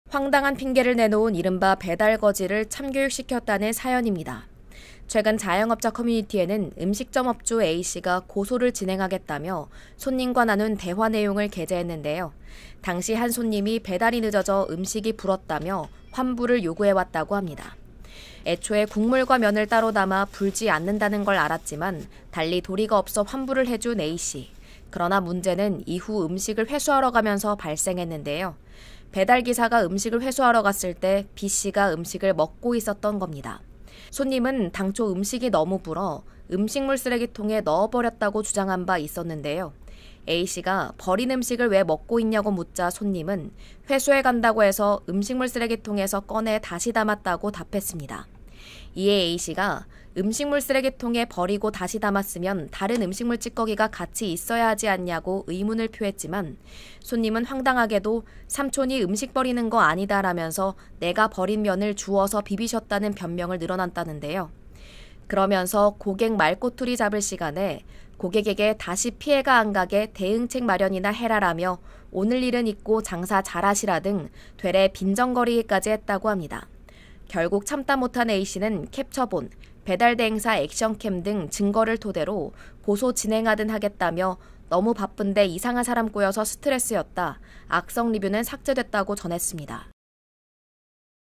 다 불어서 버렸다더니... "삼촌이 음식버리는 거 아니라며 주워왔다" / KNN
최근 자영업자 커뮤니티에는 음식점 업주 A씨가 "고소를 진행하겠다"며
손님과 나눈 대화 내용을 게재했는데요.

 / knn.news